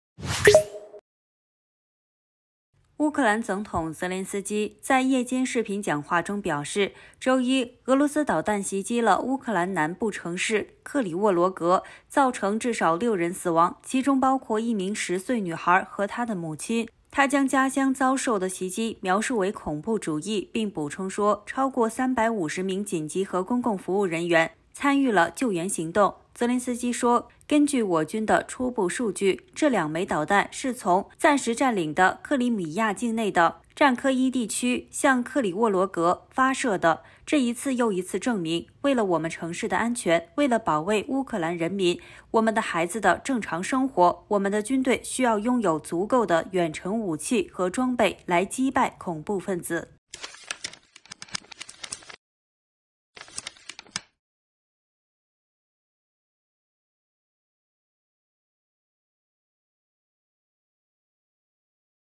俄罗斯的“恐怖主义”袭击造成6人死亡 包括10岁女孩和她的母亲
乌克兰总统泽连斯基在夜间视频讲话中表示，周一俄罗斯导弹袭击了乌克兰南部城市克里沃罗格，造成至少 6 人死亡，其中包括一名 10 岁女孩和她的母亲。他将家乡遭受的袭击描述为“恐怖主义”，并补充说，超过 350 名紧急和​​公共服务工作人员参与了救援行动。泽连斯基说“根据我军的初步数据，这两枚导弹是从暂时占领的克里米亚境内的占科伊地区向克里沃罗格发射的。这一次又一次证明，为了我们城市的安全，为了保卫乌克兰人民、我们的孩子的正常生活，我们的军队需要拥有足够的远程武器和装备来击败恐怖分子”